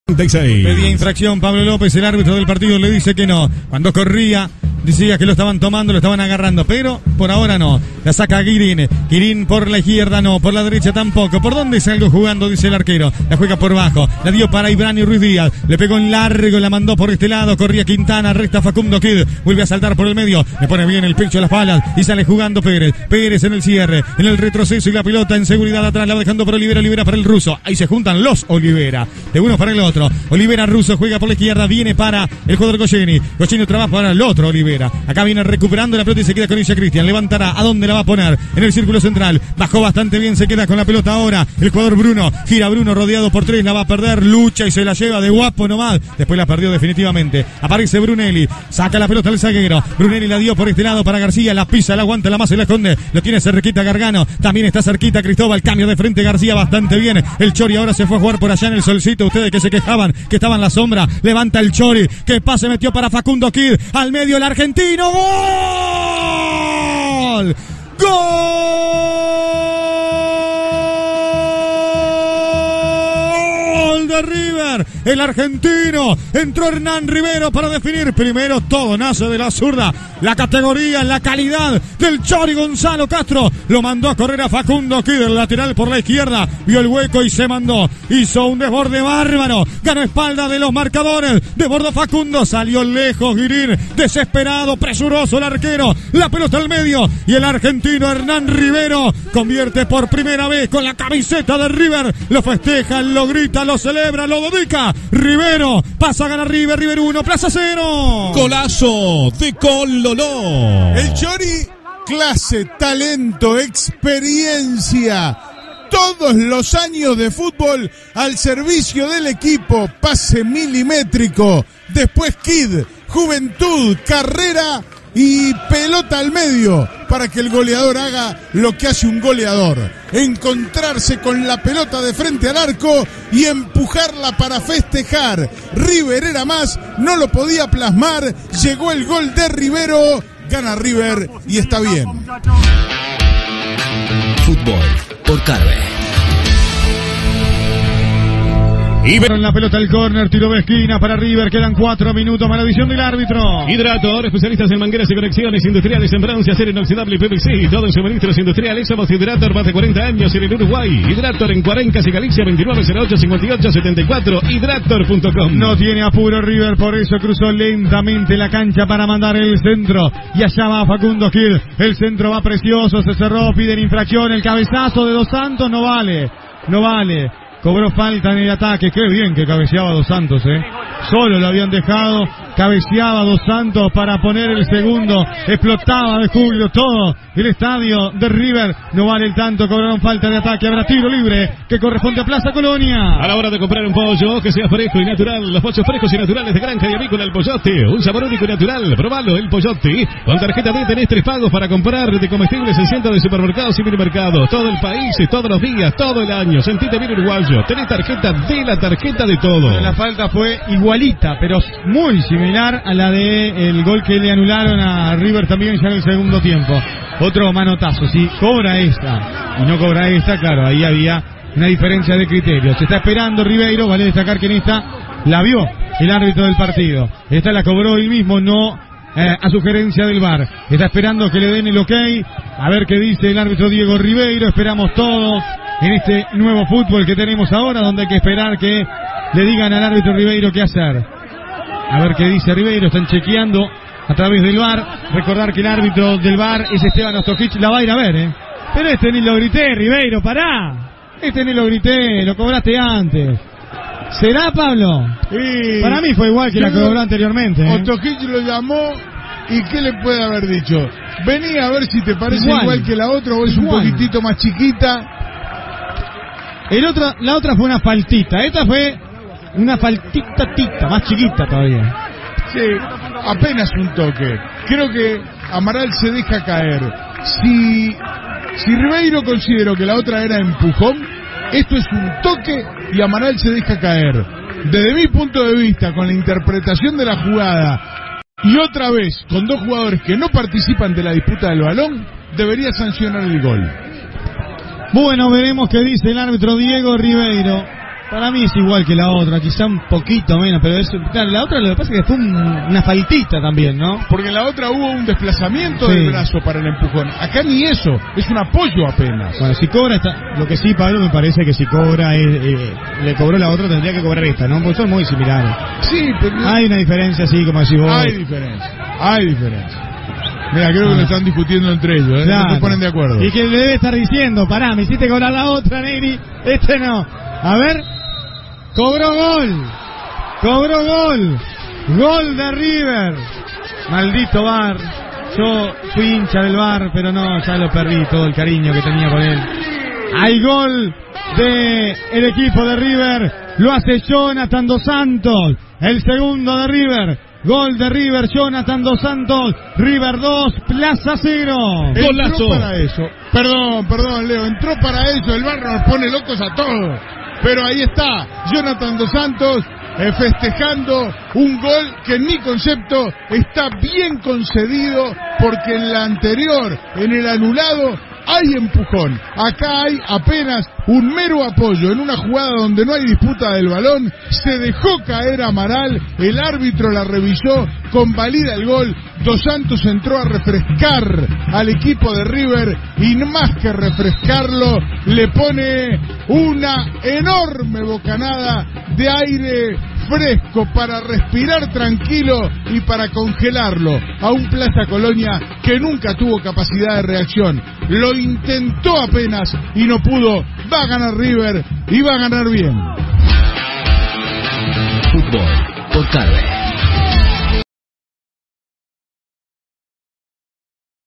River 2 Plaza Colonia 0 - Apertura 2023 (por Daniel Alonzo)
Los goles del partido, por Daniel Alonzo y Pablo Karslian para CX 24 Carve Deportiva 1010 AM.